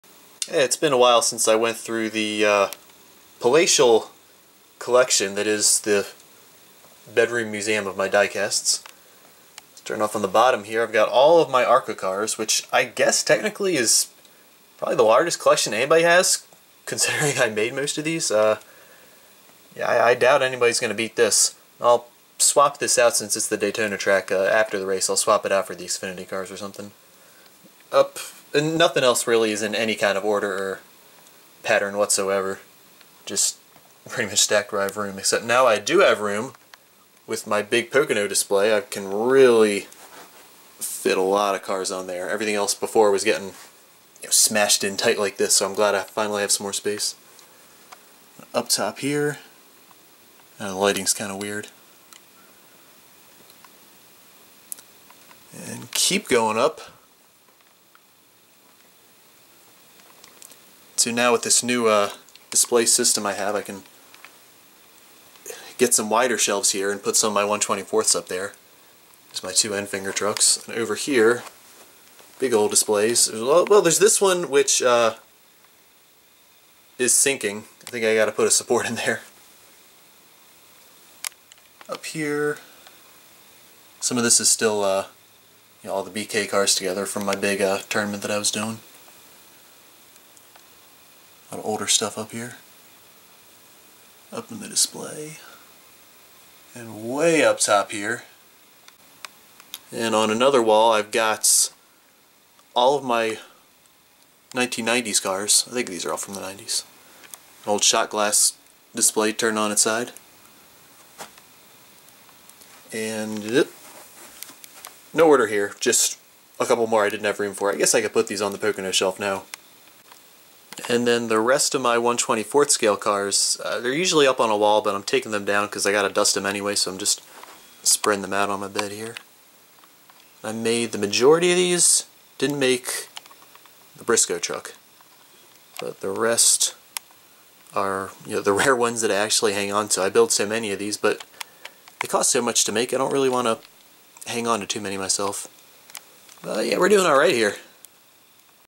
ARCA/NASCAR Diecast Collection (Feb 2019)
Update! I haven't shown off my full display in a while, so here's a quick tour of the walls of my totally-grown-up bedroom. (BIG fan of Youtube ending this video's url in a gay slur. Thanks a lot fellas.)